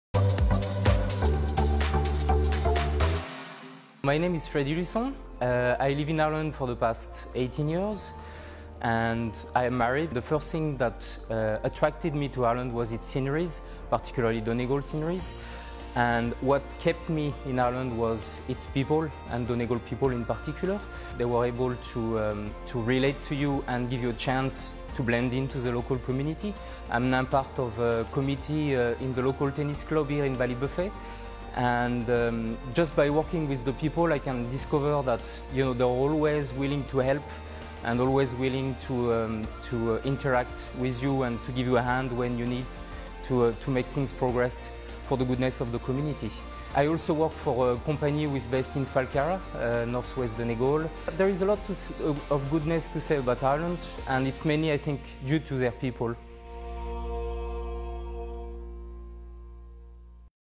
‘Gimme A Minute’ - The Immigrant Isle
Another of our 'Gimme a Minute' videos where we take a look at what draws people from other nationalities to our beautiful country.